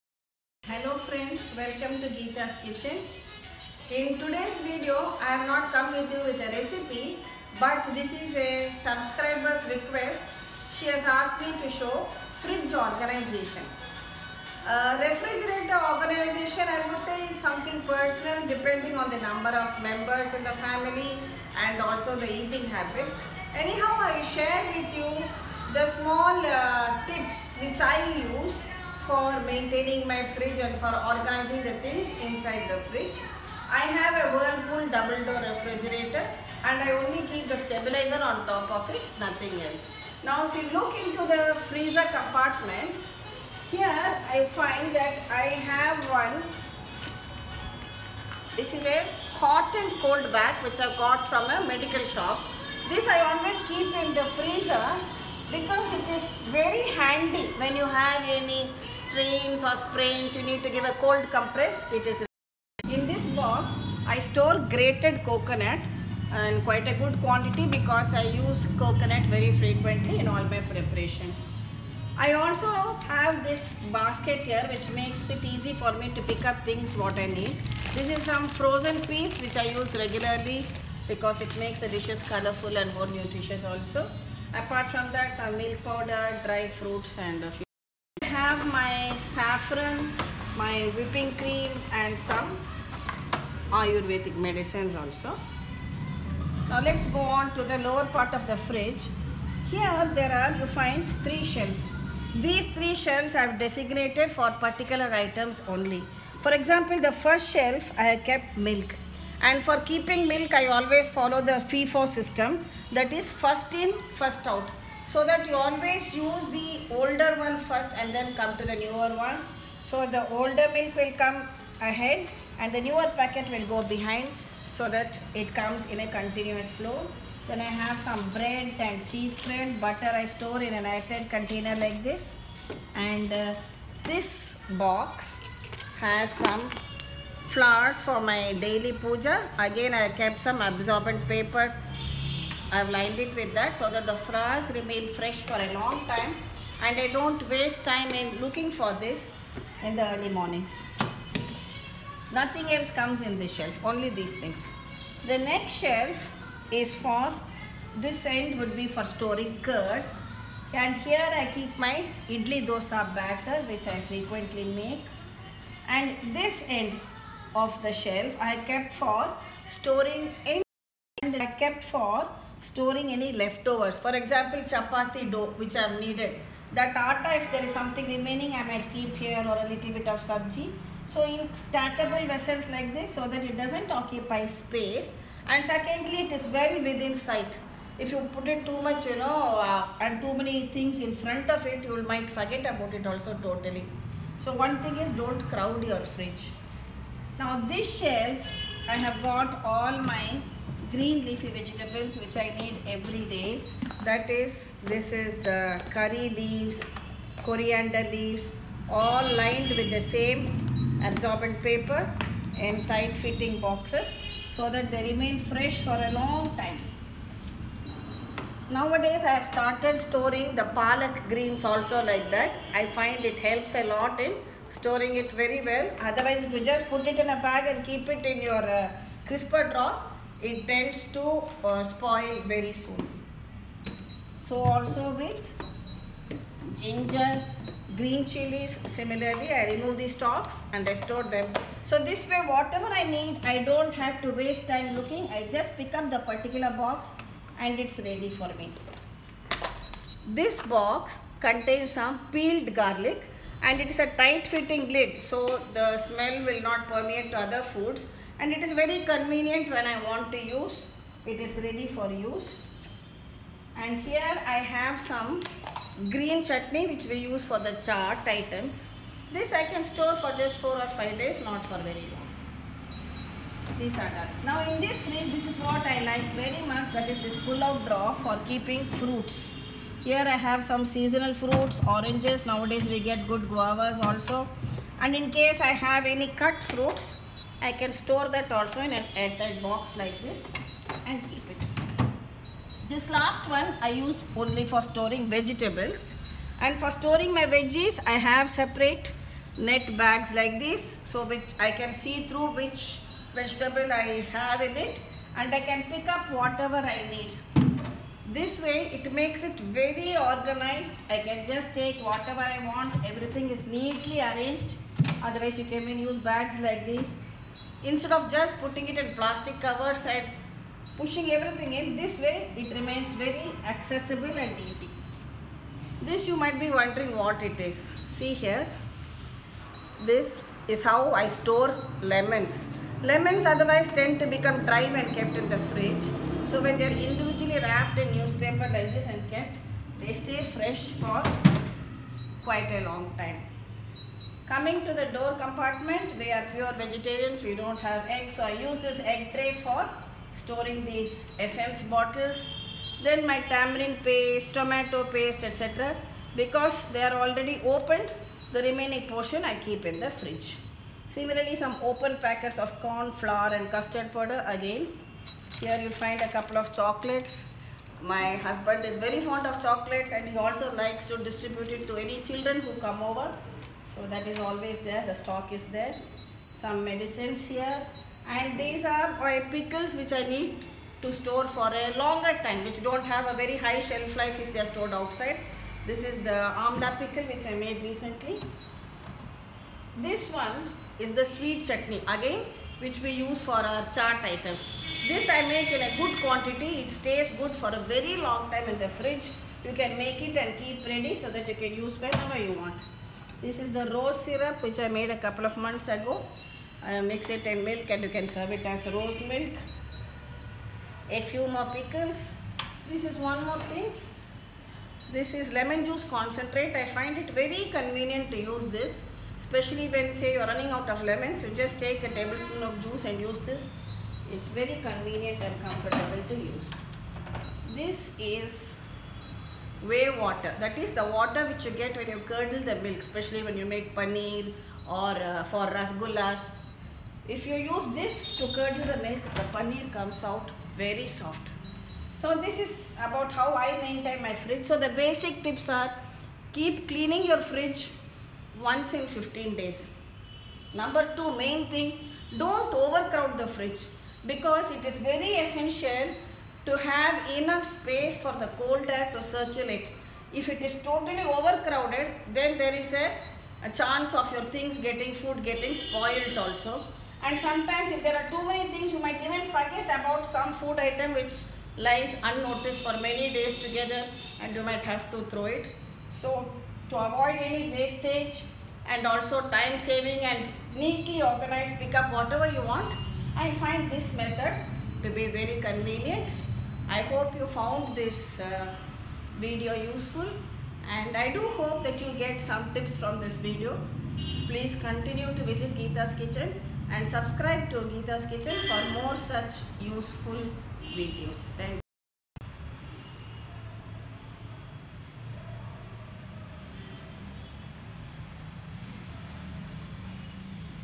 Fridge Organisation
In this video I have shown how I have organised the various items in the refrigerator.A planned systematic way of keeping things , helps in saving a lot of time. apart from that, there is no unnecessary wastage of food and the efficiency of the fridge is also optimised.